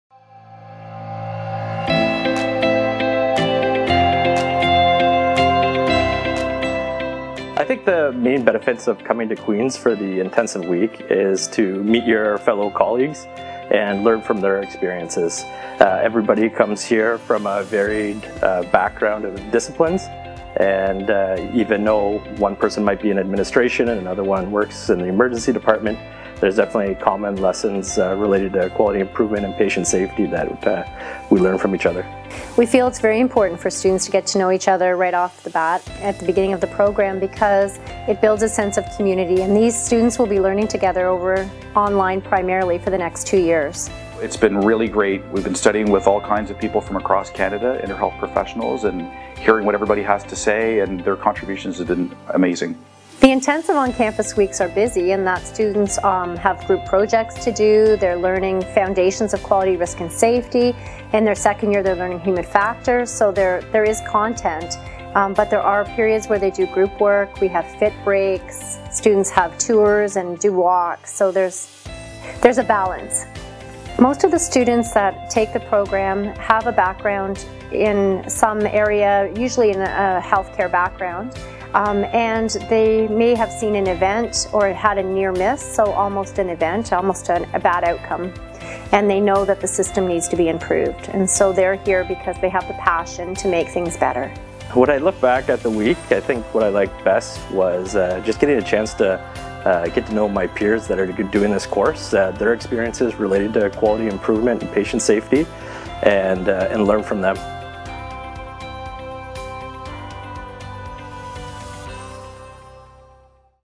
Healthcare Quality Intensive Weeks
The Master of Science in Healthcare Quality MSc(HQ) is the first of its kind, specializing in linking theory and practice in quality, risk, and safety in healthcare. Participants have two one-week intensive sessions each year and the remainder of the program is online.